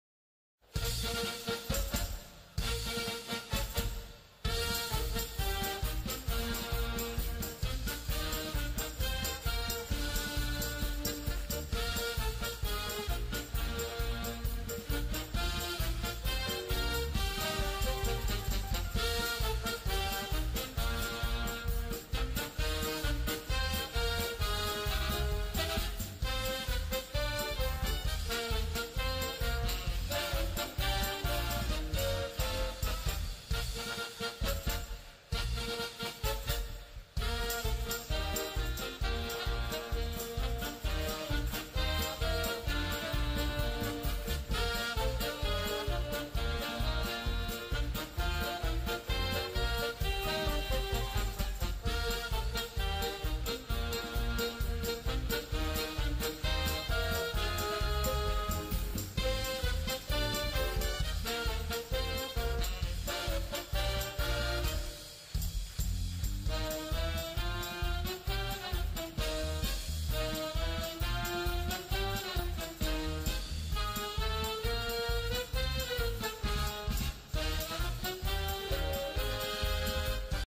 MEMES INDEPENDIENTE PETROLERO 0 PALMEIRAS 5 COPA LIBERTADORES 2022
Aquí una recopilación de los mejores memes de la victoria y goleada de Palmeiras frente a Independiente Petrolero en la Copa Libertadores 2022.